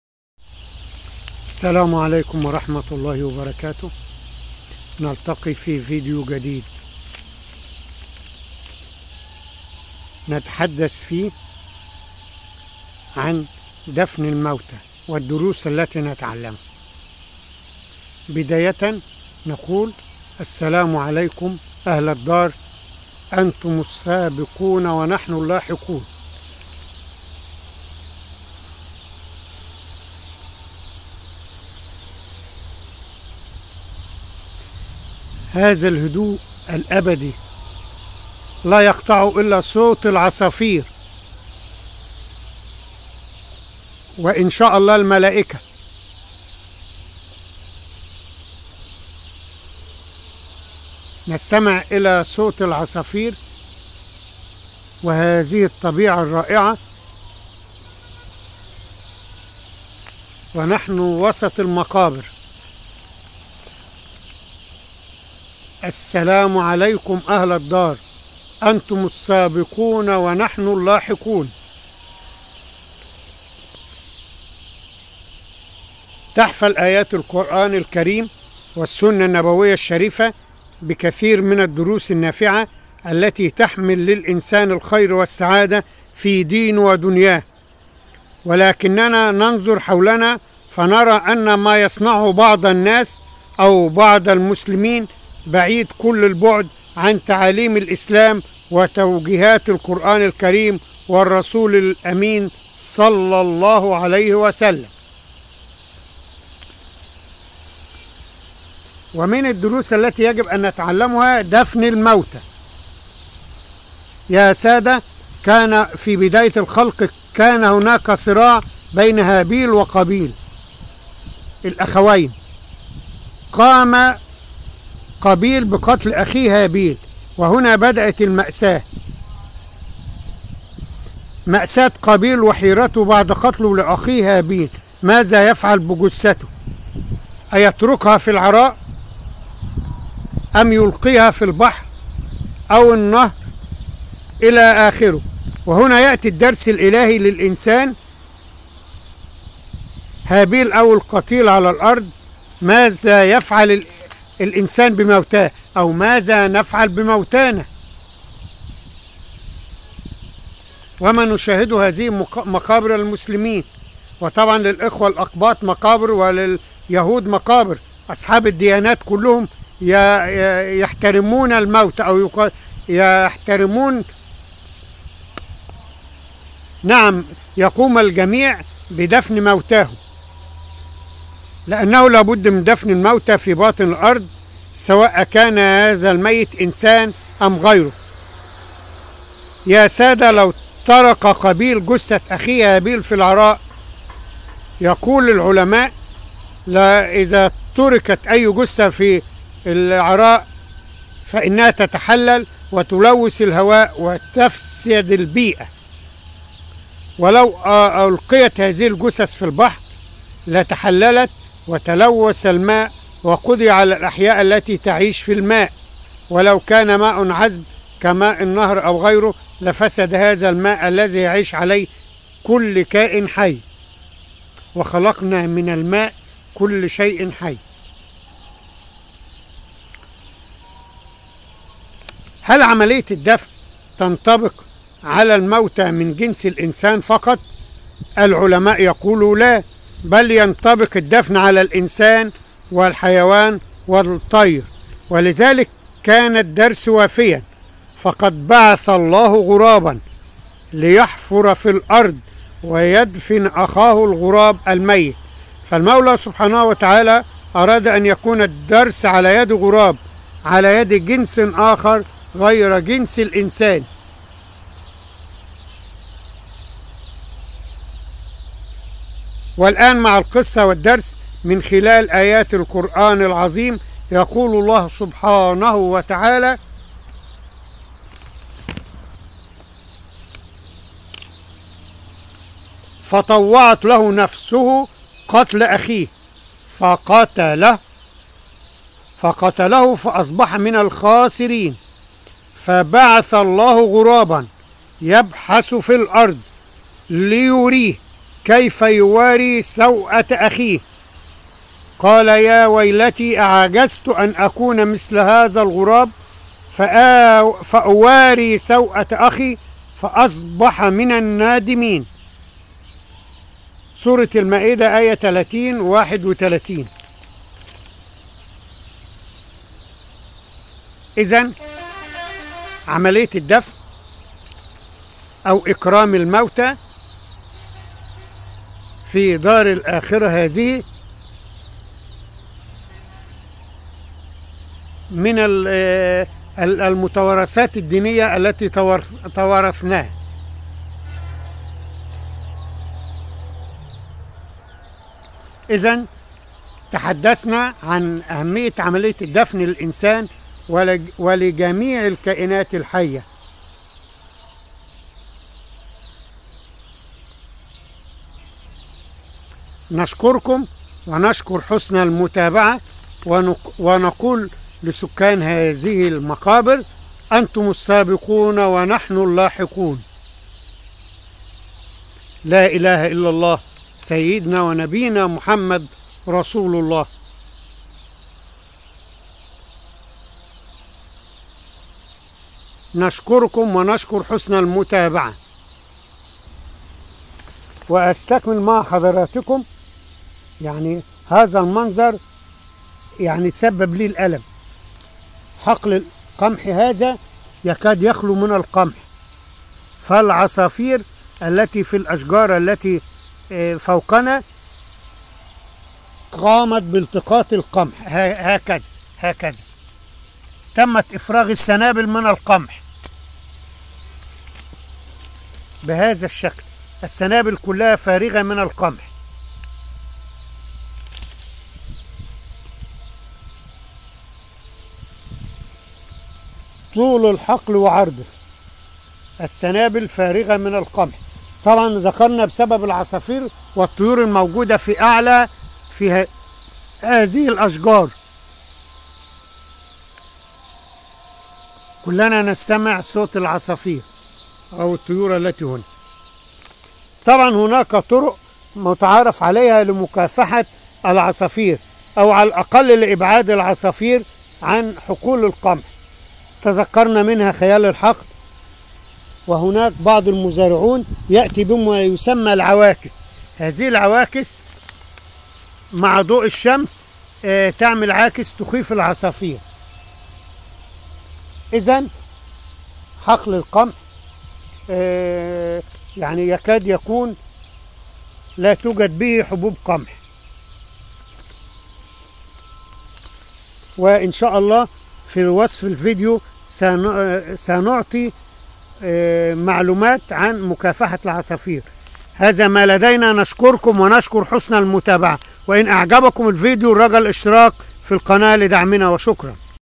ماذا فعل قابيل بأخيه هابيل ( مقابر قريتنا ودفن الموتي) توثيق،
البقلية ،، منطقة المدافن ، ،إكرام ودفن الموتي ،مشكلة قابيل ،وهابيل  ،  ،  ( اعجرت أن أكون مثل هذا الغراب ،فاواري سوءة اخي ) ،